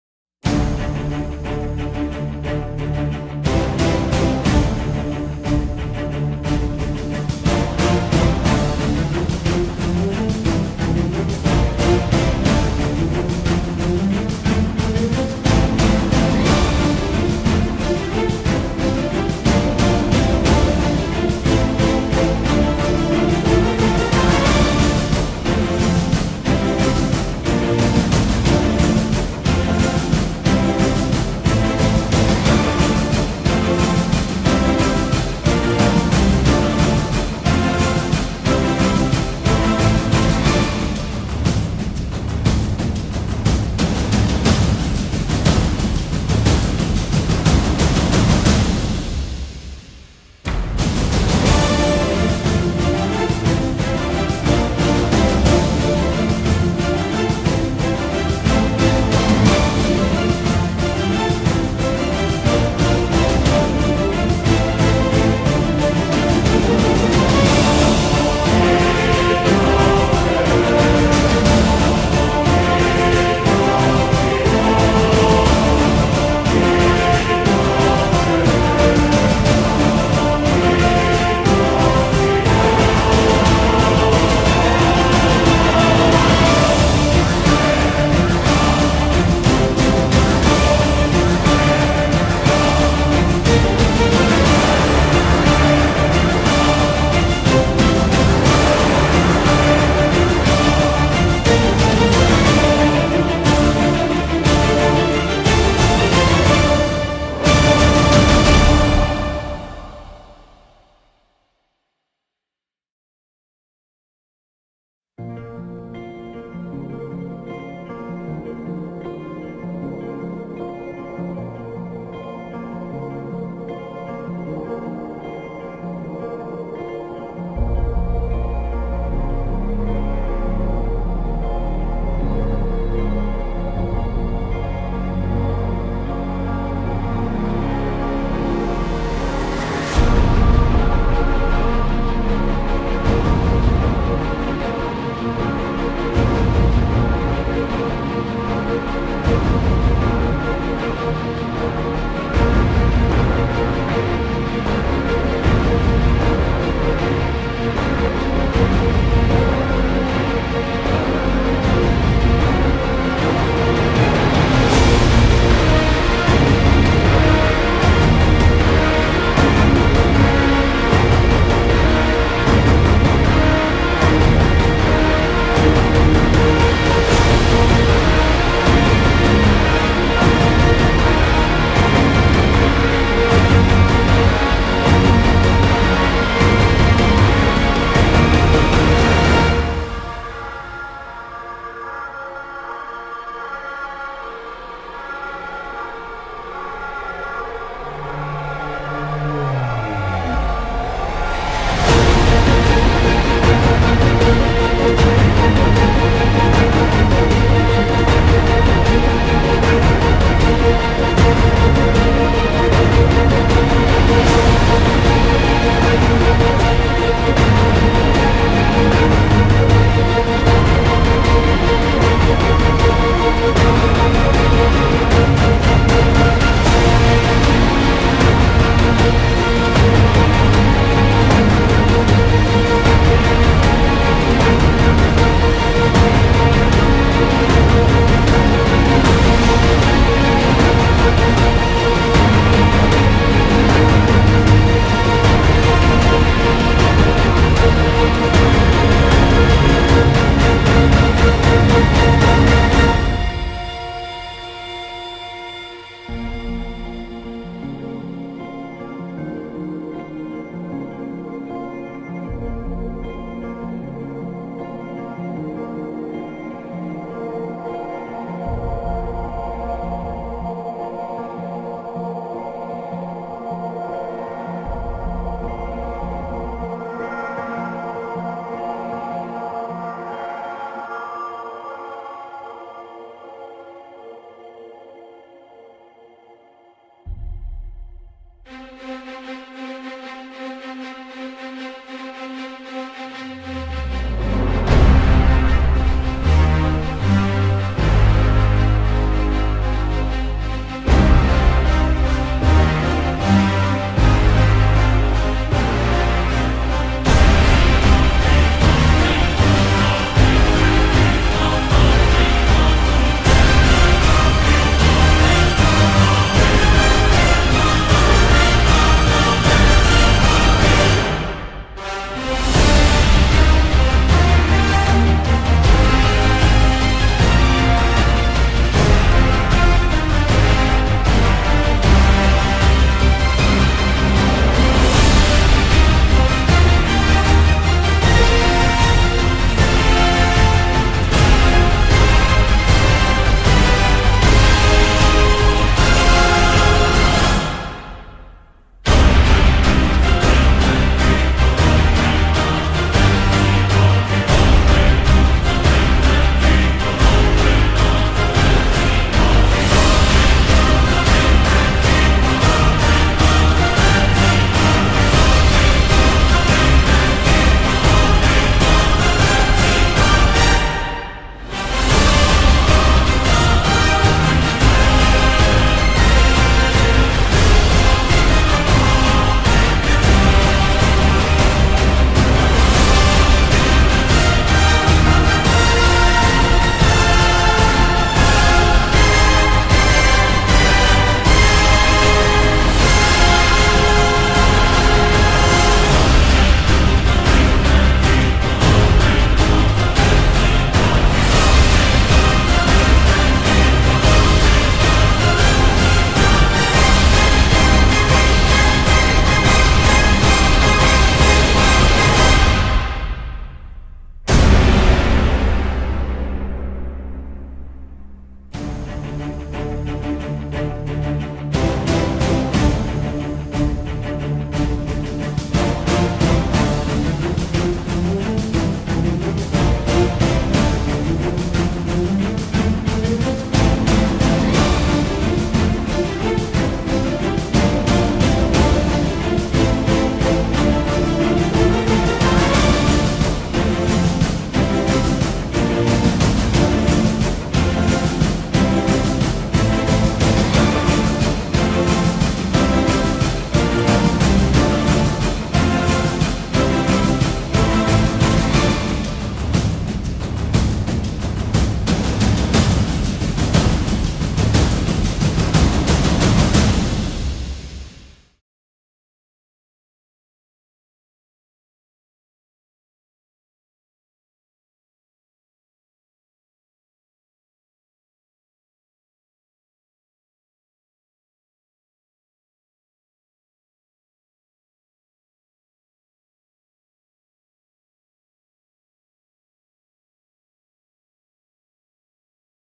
Drone Flies Through Fireworks Over Castle Cornet Guernsey
Drone through fireworks over Castle Cornet in Guernsey